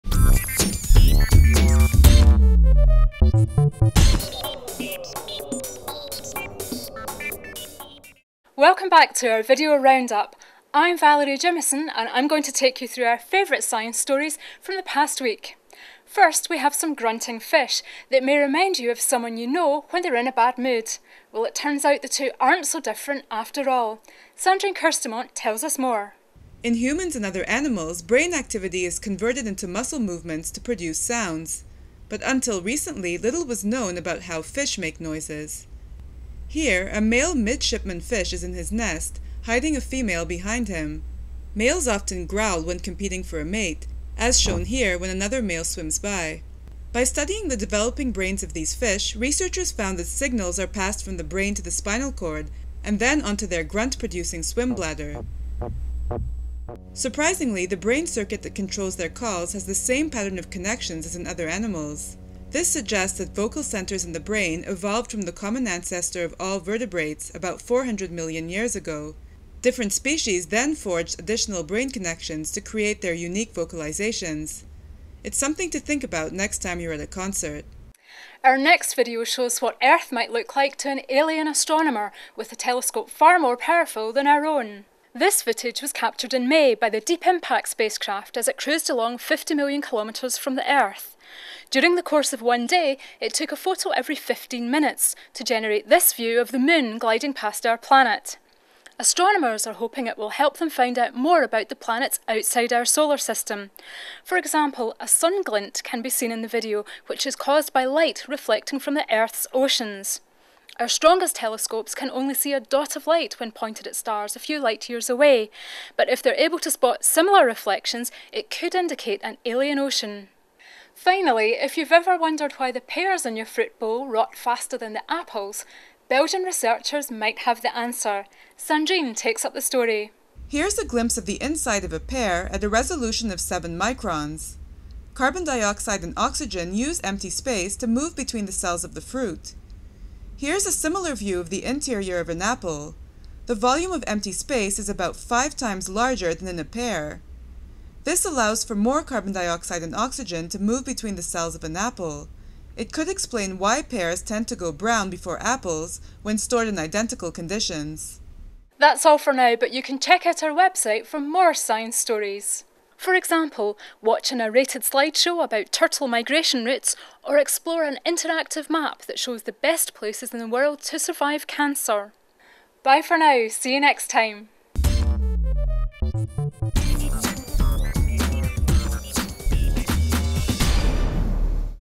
New Scientist video round-up - July 18, 2008
Find out what human speech has in common with fish grunts

See a view of the Earth captured from 50 million kilometers away